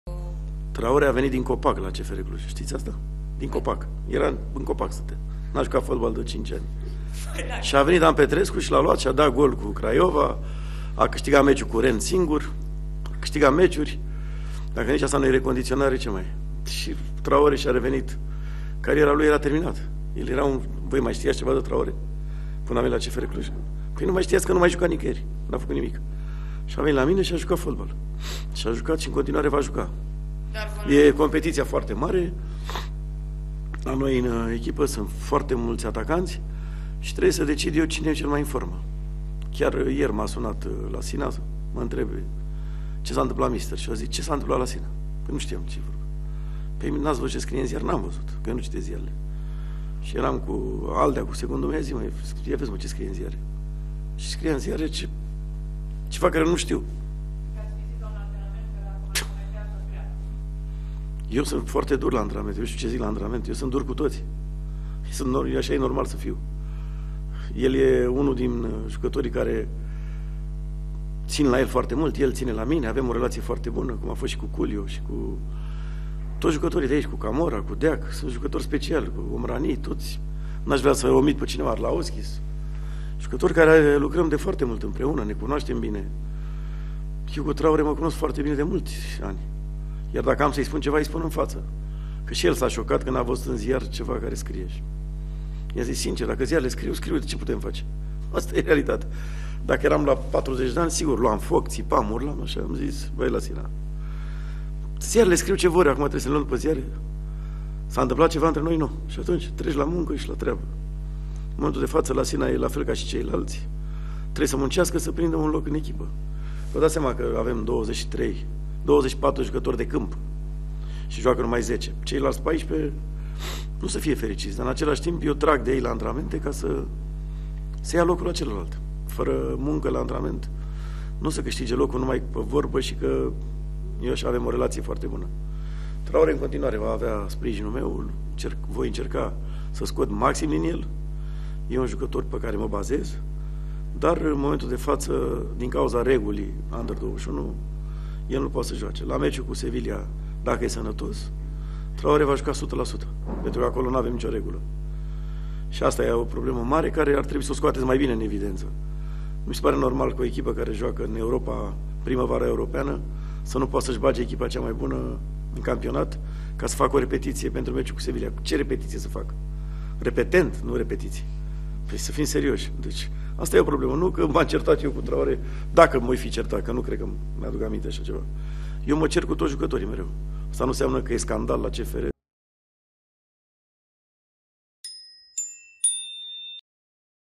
Dan Petrescu, declarații incredibile despre Traore: „Stătea în copac înainte să vină la CFR Cluj!”
Dan Petrescu, antrenorul celor de la CFR Cluj, a susținut o conferință de presă înaintea meciului cu Viitorul, de vineri, ora 20:00. Tehnicianul a vorbit despre atacantul ivorian Lacina Traore (29 de ani) și a spus că nu are nicio problemă cu acesta. Din contră, Petrescu spune că va miza pe el în meciul cu Sevilla, din Europa League.